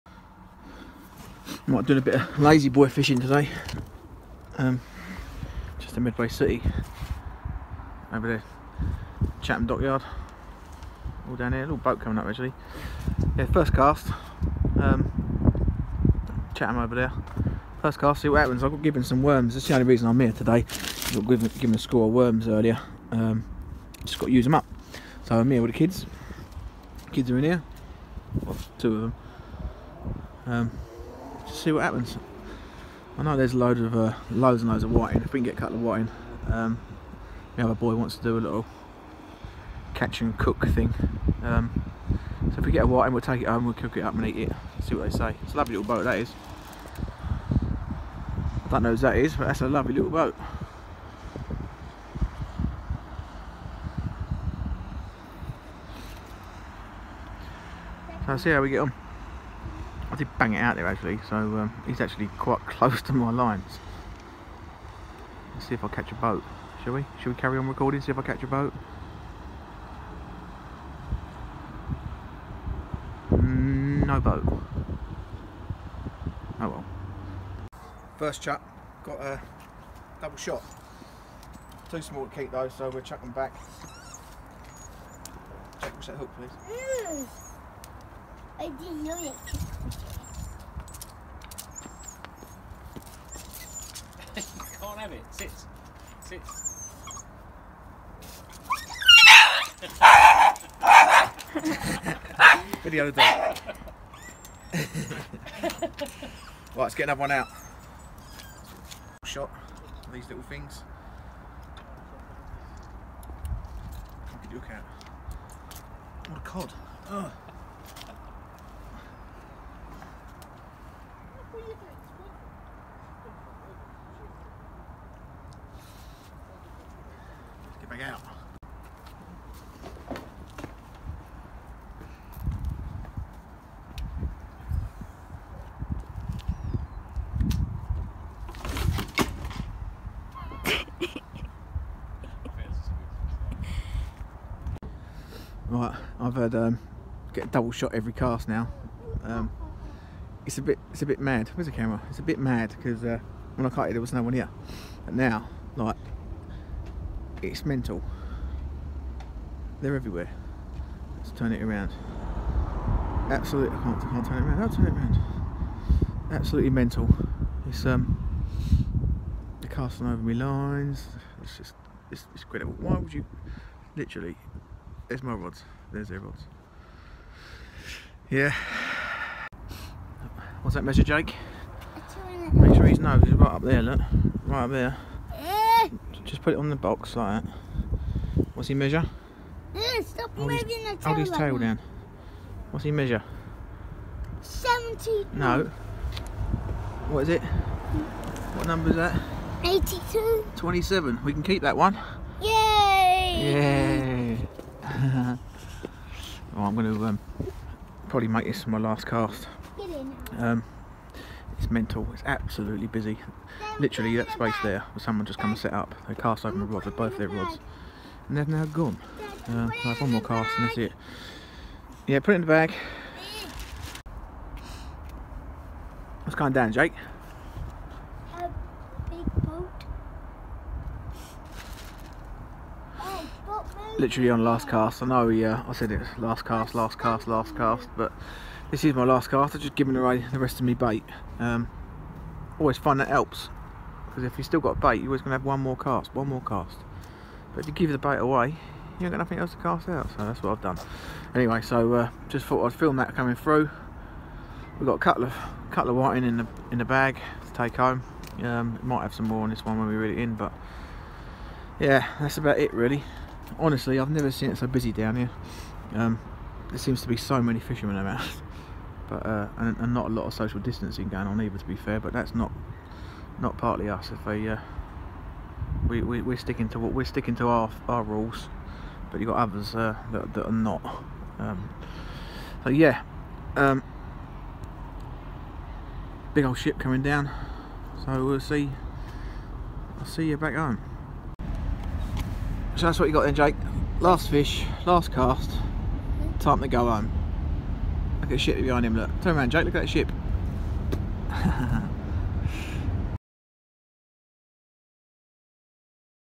Whiting fishing at Medway City Estate, winter sea fishing mce Kent
Got given a score of lug so grabbed the rod and the kids see if we could catch a few